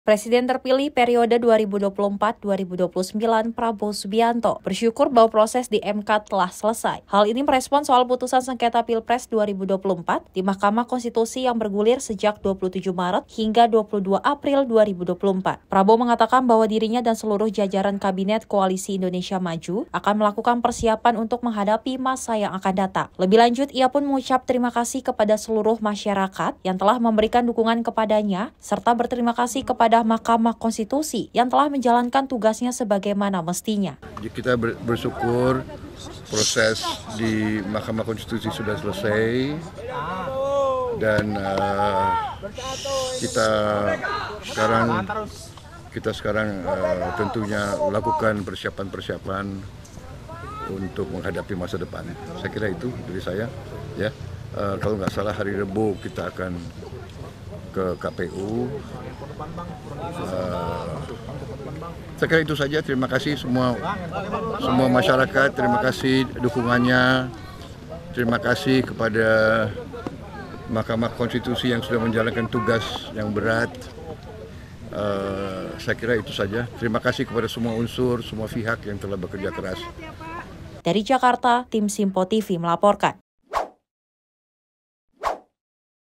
Presiden Terpilih, Bersyukur Atas Kemenangan di MK
Usai pembacaan putusan oleh Mahkamah Konstitusi  terkait sengketa Pilpres 2024 yang berlangsung pada senin kemarin. Presiden terpilih Prabowo Subianto  mengaku bersyukur atas proses persidangan sengketa Pilpres 2024 telah selesai. Ia mengatakan setelah putusan MK pihaknya akan bersiap untuk menghadapi masa depan.